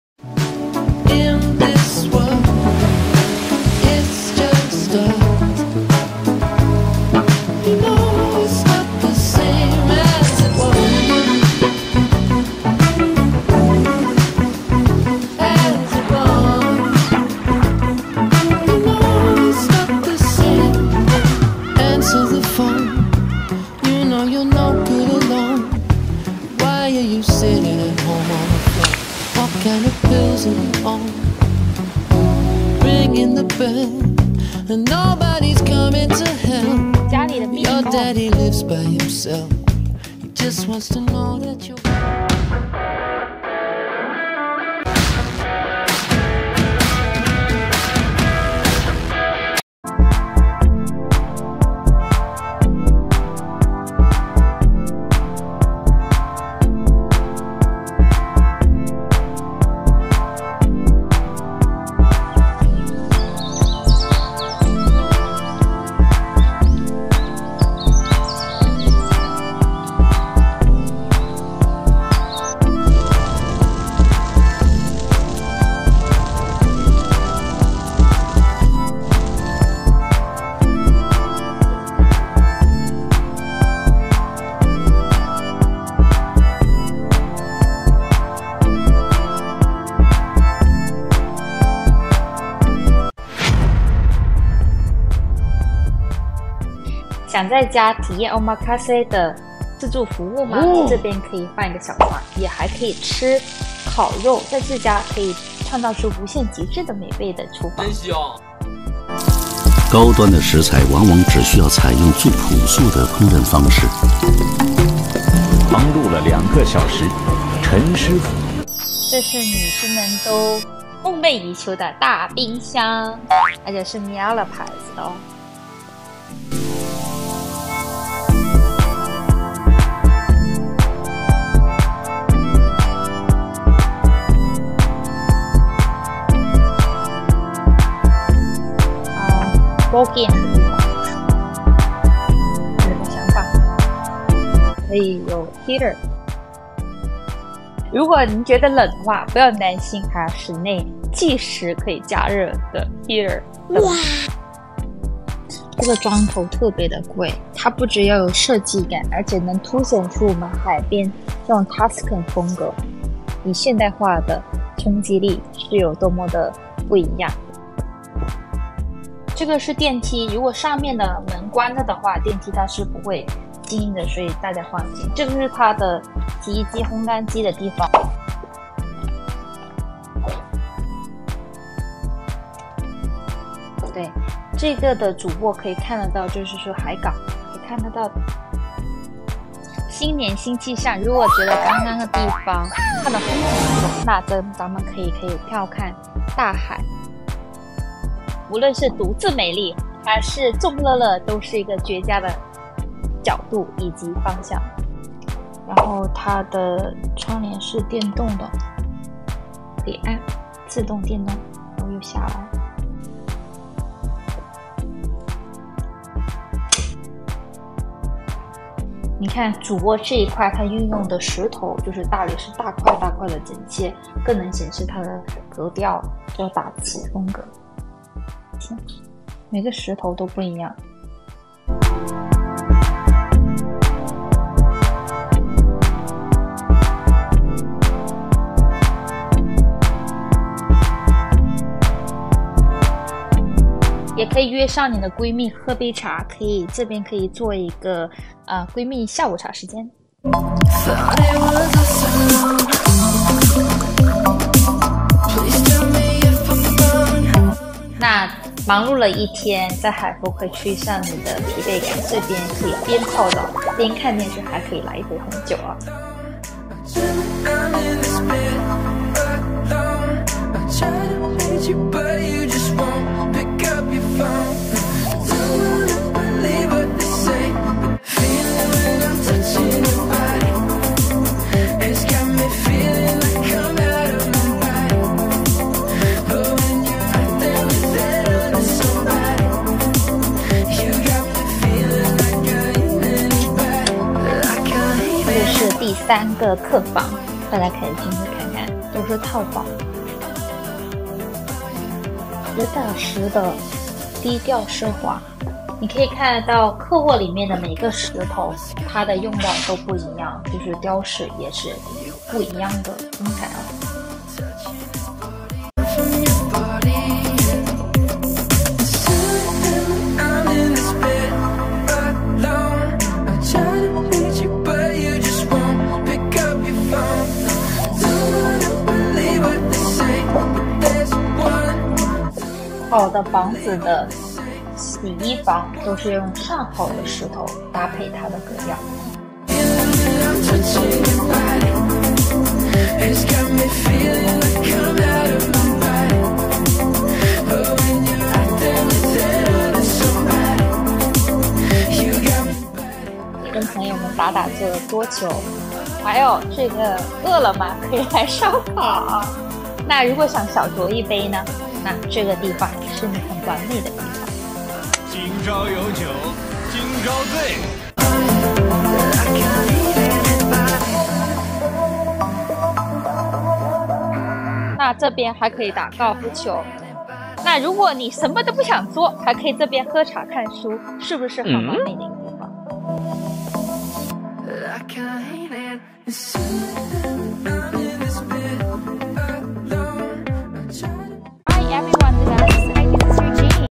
【Mansion Collection】Bidding Price Starting at $6,598,000. Address 358Dahia Place Corona Del Mar
BID March 9-16th. Listed for $6.598M.  3 beds 5bath, 3,130 Sqft. Contemporary luxury and timeless views make for a perfect pairing in 358 Dahlia Place. This newly built condominium in exclusive Corona del Mar is sure to become your ideal destination retreat.
全新美房，3房5卫。上市价格$6,598,000. 竞价时间3/9-3/16. 眺望海景，时尚高档康斗，应有尽有。独乐乐或众乐乐，尽情享受南加州新港城最美的海岸线。在家里可以享受与友人相伴下午茶或者在高档的厨房里享有Omakase 自助美食，也可以来几下庭院高尔夫球，人生的梦想便是在这此足已